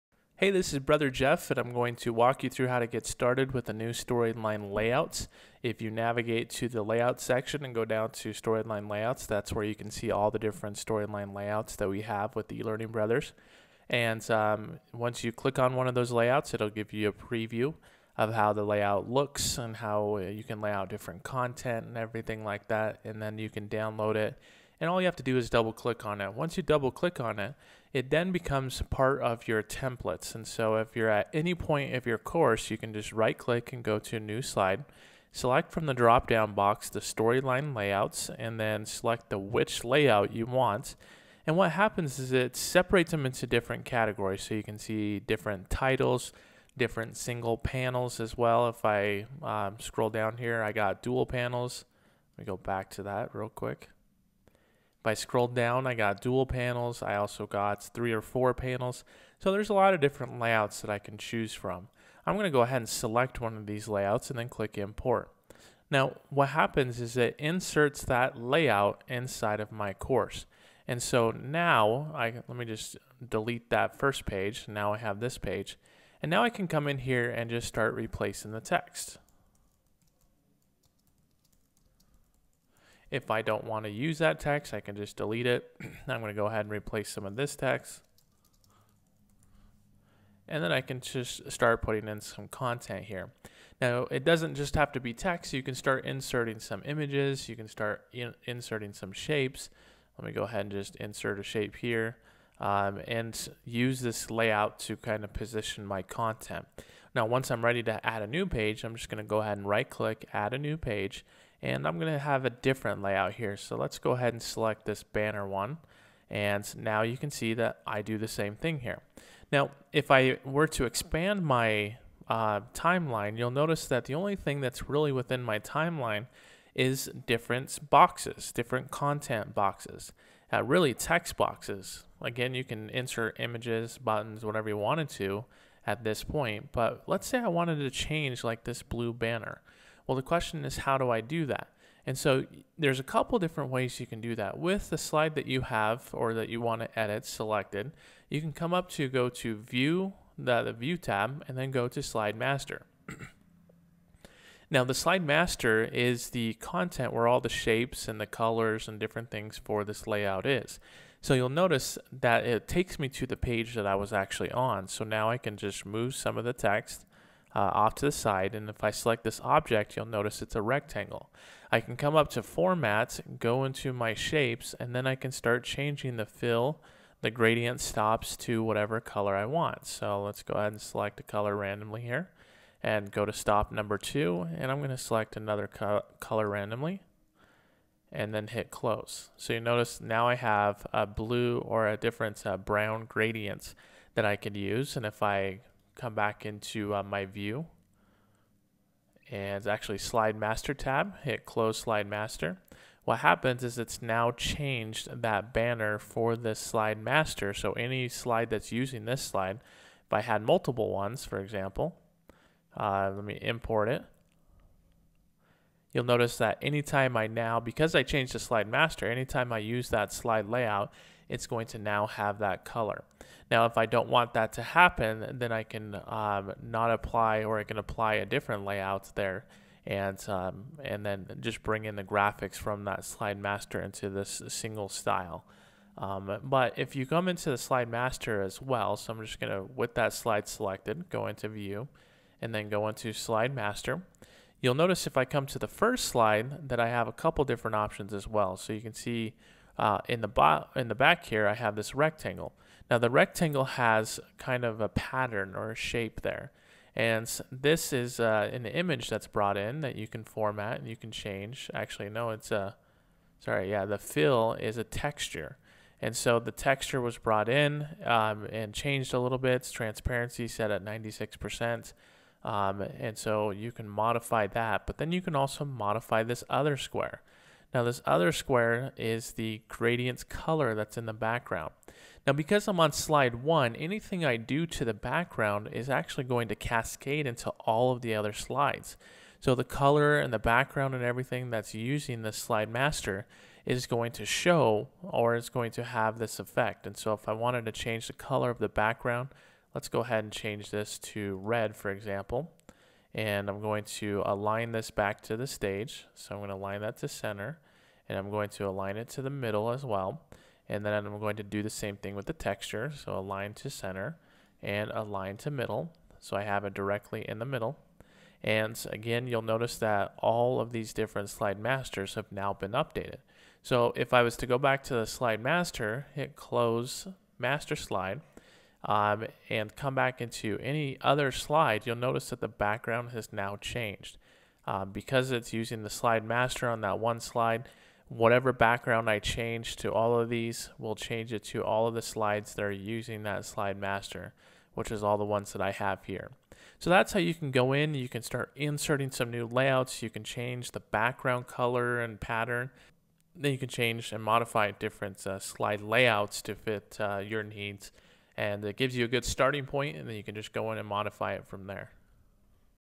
How to Edit the Storyline Layouts
In this short tutorial Brother Jeff will show you how to get started with the new eLearning Brothers Articulate Storyline Layouts. You will also learn how to customize and tweak the look and feel of each layout.